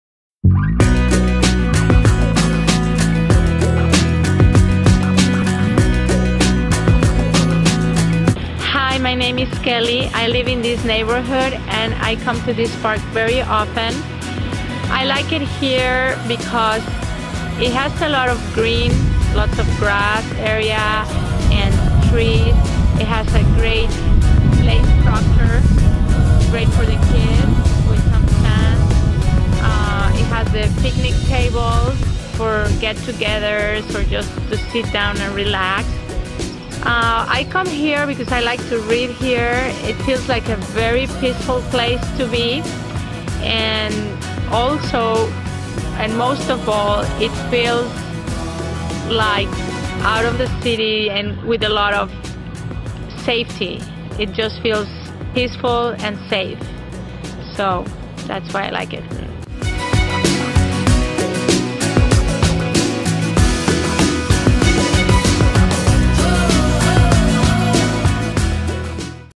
Solana Highlands School Park | Carmel Valley San Diego 92130
The Solana Highlands School Park is a quality park in the Carmel Valley community that goes to show the teamwork in our community with the school district and the city of San Diego. Located on the very peaceful and quiet corner of Bernwood Place and Long Run Drive this park has a safe and peaceful atmosphere, which is great for parent with kids. The eye engaging three-structure playground can be a haven for kids (and their parents). Equipped with a giant sand box, slides, swings, monkey bars, and a lot more.

There are several picnic areas, with their own charcoal grills, where one can organize events or just have a quality family outing. These picnic areas are scattered on a large green field where dogs and kids can be found running around. This park is a symbol of our great community and there are many other parks like it in Carmel Valley.